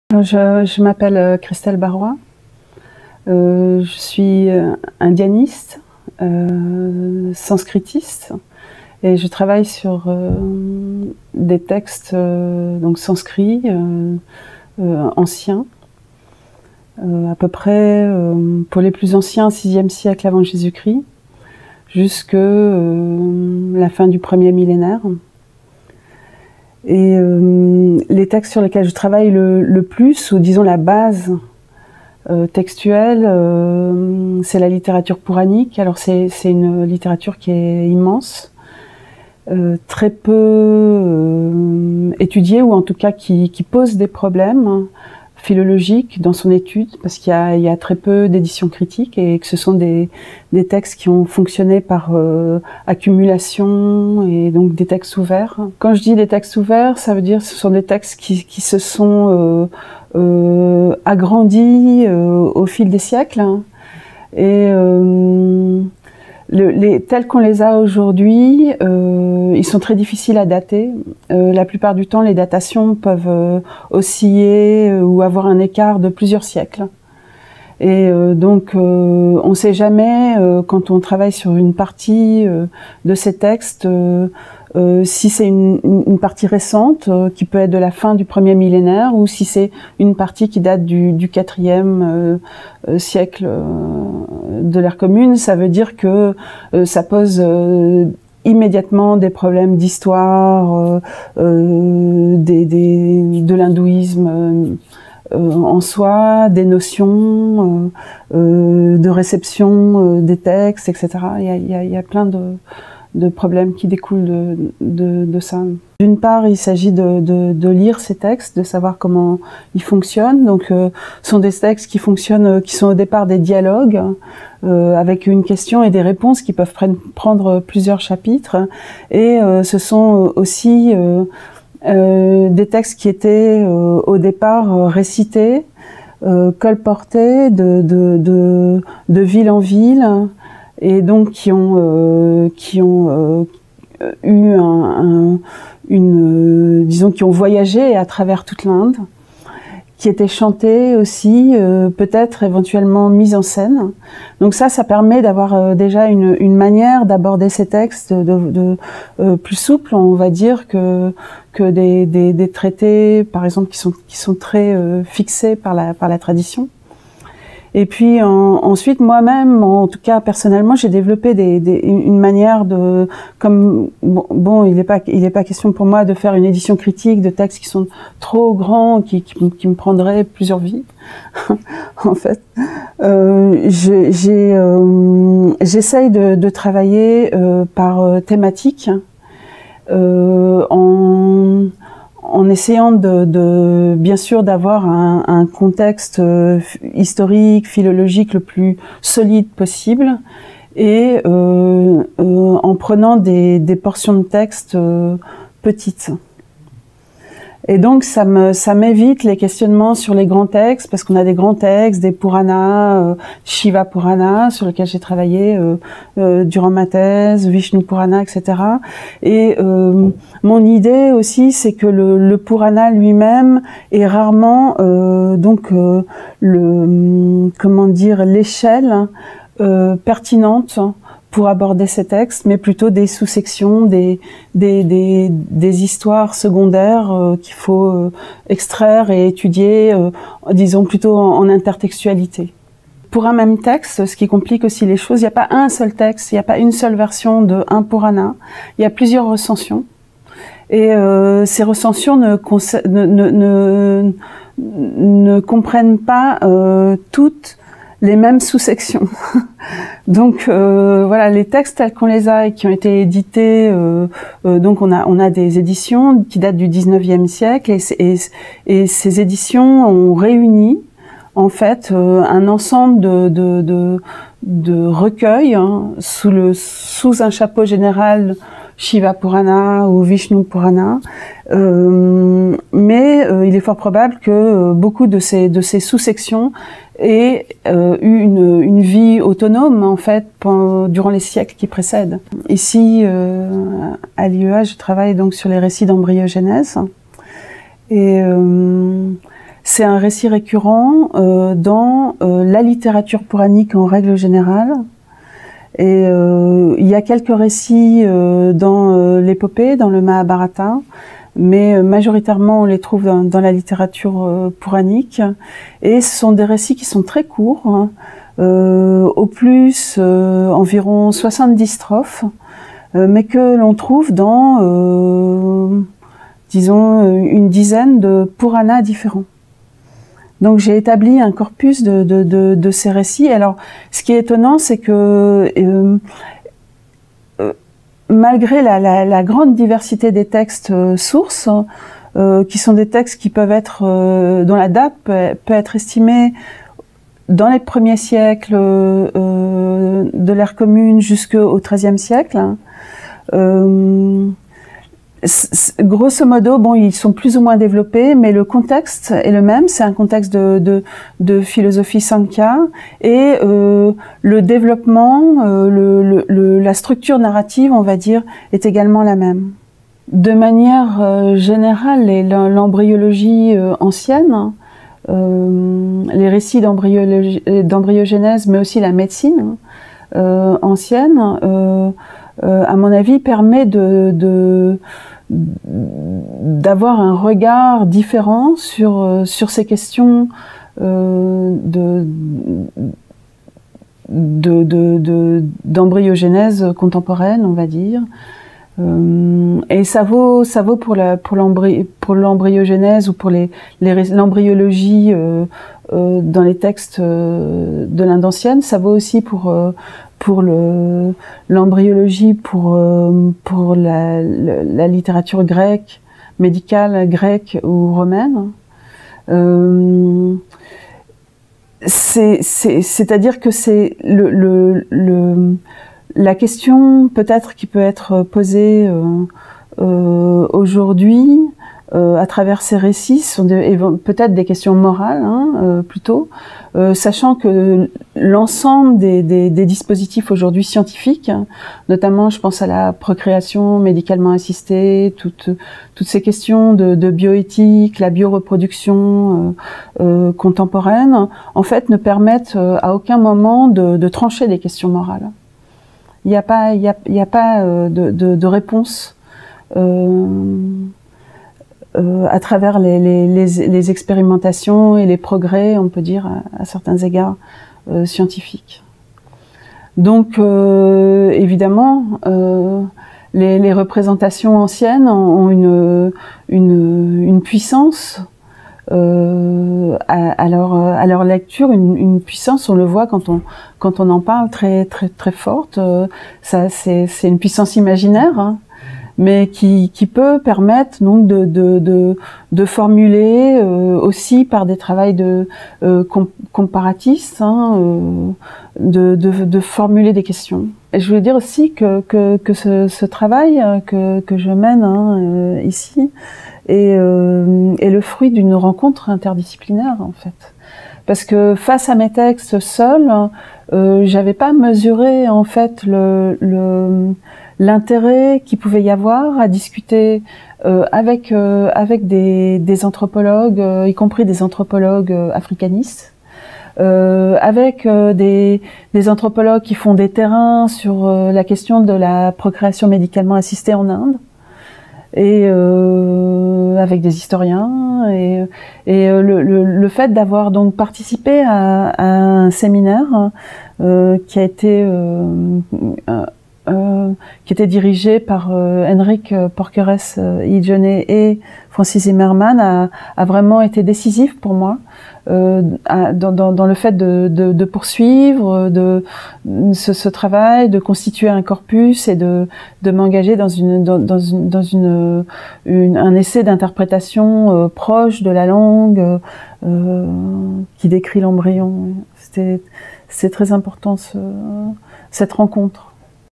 Christèle Barois - Le récit d’embryogenèse dans les sources sanskrites médiévales
Le récit d’embryogenèse dans les sources sanskrites médiévales et l’histoire de son exégèse (XIIIe-XIXe s.)

L’étude des représentations de l’embryon et du fœtus humains est un champ heuristique des représentations sociales dans leur ensemble, qui suscite des intérêts appuyés dans les disciplines de l’anthropologie historique, de l’histoire des sciences et de la philosophie, à des époques historiques s’échelonnant de l’Antiquité classique à aujourd’hui. Toutefois, le récit du développement embryonnaire dans la tradition de l’hindouisme est peu étudié, car il n’apparaît pas comme un locus classicus de la doctrine et n’a pas acquis pour lui-même le statut d’autorité scripturaire. Le projet « Le récit d’embryogenèse dans les sources sanskrites médiévales et l’histoire de son exégèse (XIIIe-XIXe s.) » a pour objet l’étude des récits du développement embryonnaire dans le Mahābhārata et les Purāṇa. Grâce à l’élaboration d’un corpus spécialisé de récits récurrents, la traduction et l’analyse de ces récits, l’examen de leurs liens intertextuels, l’identification de leurs sources philosophiques et médicales, ce projet vise à étudier les représentations de l’embryon humain dans l’hindouisme ancien et médiéval et à mettre en évidence les enjeux théologiques qui président à ces représentations.

Biographie

Indianiste, sanskritiste, Christèle Barois a séjourné en Inde durant quatre années (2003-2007) en tant qu’allocataire de recherche au sein du département d’Indologie de l’Institut français de Pondichéry. Sa thèse, soutenue en 2012 à l’École Pratique des Hautes Études (EPHE), Section des sciences religieuses (Paris), est consacrée à l’étude de la Vāyavīyasaṃhitā, texte śivaïte composé en Inde du Sud vers le XIe siècle, qui couvre plusieurs champs de recherche (cosmogonie, mythologie, doctrine śivaïte, rituel, yoga). En 2015, elle a rejoint le projet ERC AyurYog : « Medicine, Immortality, Moksha: Entangled Histories of Yoga, Ayurveda and Alchemy in South Asia » (ayuryog.org), à l’Université de Vienne (Institut für Südasien-, Tibet- und Buddhismuskunde). Dans ce cadre, elle a engagé l’étude d’un texte inédit, la Dharmaputrikā Saṃhitā, probablement le plus ancien manuel de yoga qui nous soit parvenu (vers le VIIe s.).  Ses recherches actuelles portent sur les sources médicales des techniques du yoga ancien et sur les récits récurrents d’embryogenèse dans la littérature sanskrite (Mahābhārata et Purāṇa). Elle a présenté ses travaux lors de nombreux colloques internationaux, notamment les Word Sanskrit Conferences de Helsinki (2003), de Kyoto (2009) et de Vancouver (2018), et la Dubrovnik International Conference on the Sanskrit Epics and Purāṇas (2005 et 2011).